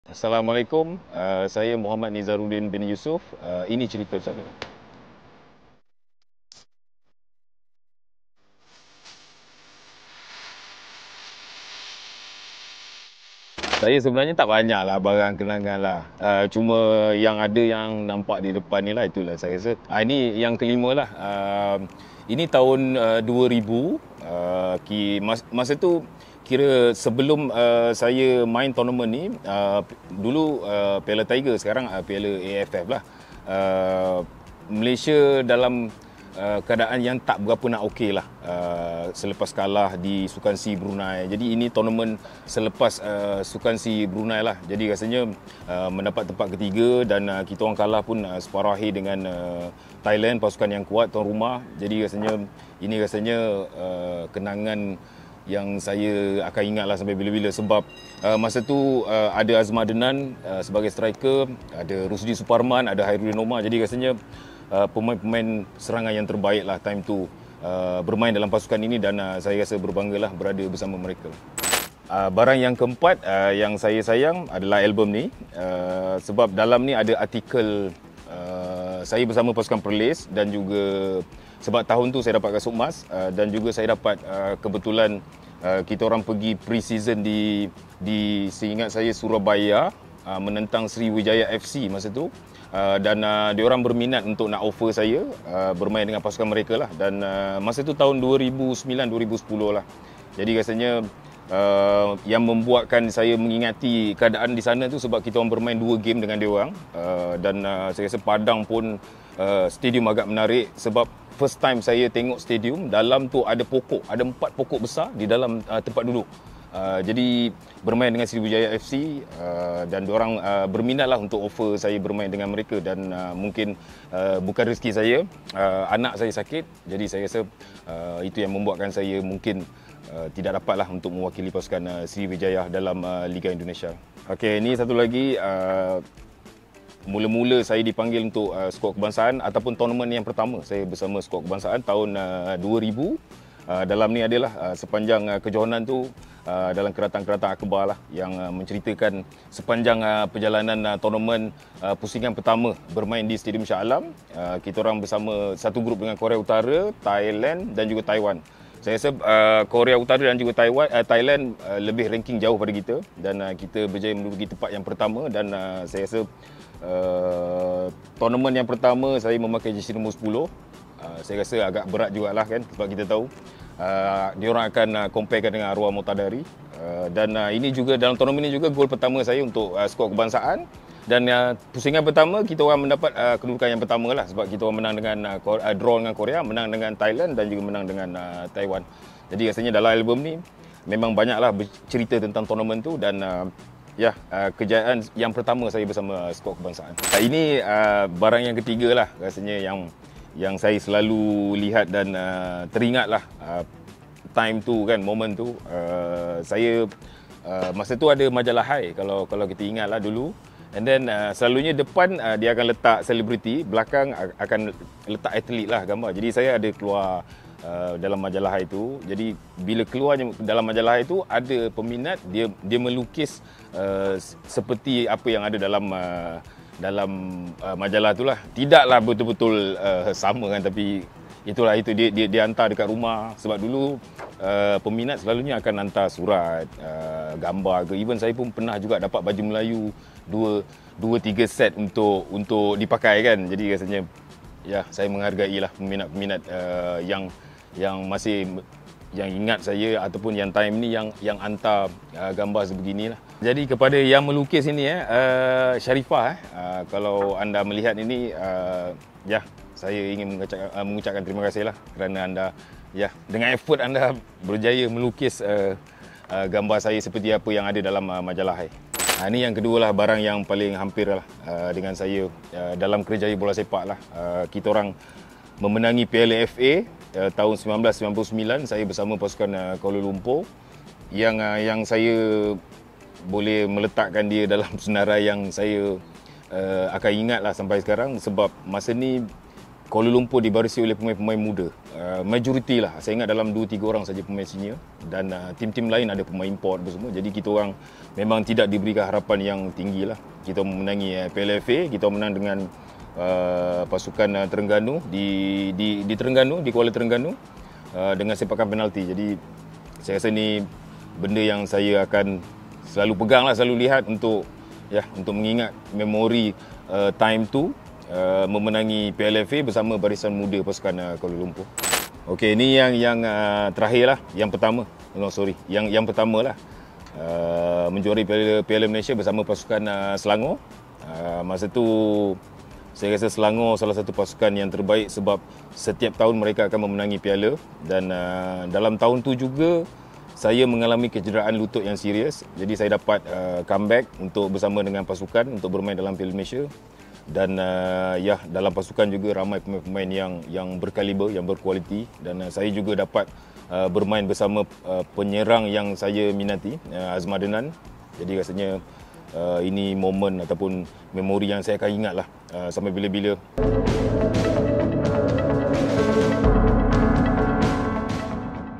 INI CERITA SAYA - NIZARUDDIN YUSOF
Wah tak sangka Nizaruddin Yusof masih simpan lukisan daripada peminat waktu dia muda dahulu.

Apa lagi barang menarik yang menjadi kenangan untuk bekas pemain kebangsaan ini?

Jom kita tengok